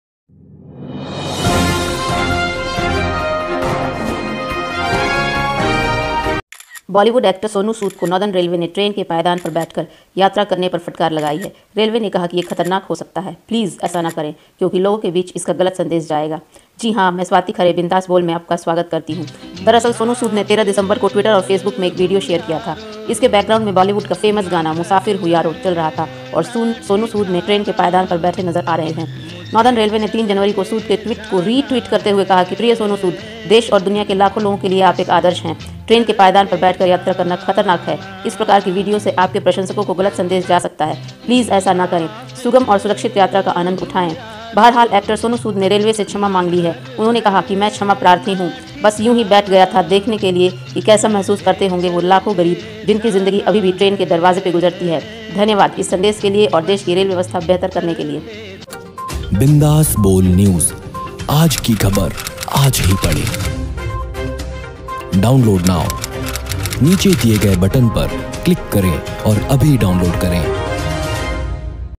किस मजबूरी में बॉलीवुड एक्टर ने ट्रेन के गेट पर लटककर किया सफर, फिल्म एक्टर सोनू सूद ने मांगी माफी
बिंदास बोल के साथ देखिए देश- विदेश की सभी महत्त्वपूर्ण और बड़ी ख़बरें l